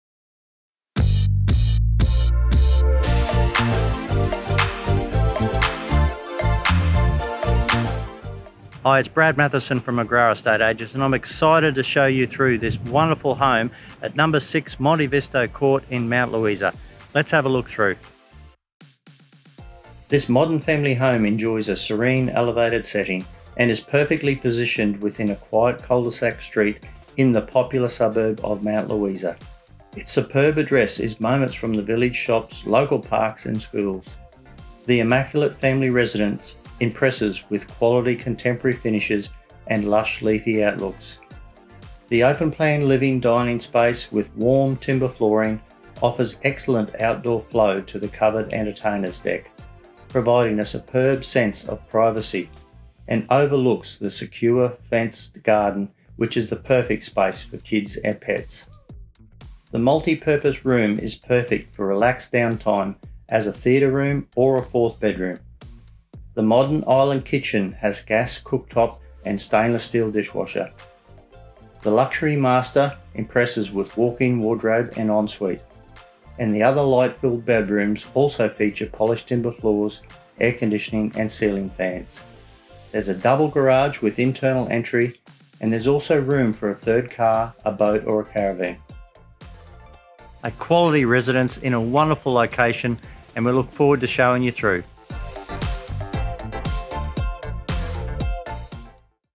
6 Monte Visto Court, Mount Louisa
Perfectly positioned on an elevated parcel within a quiet cul-de-sac street, this immaculate family residence impresses with quality contemporary finishes and lush leafy outlooks. Its superb address is moments from village shops, local parks and schools.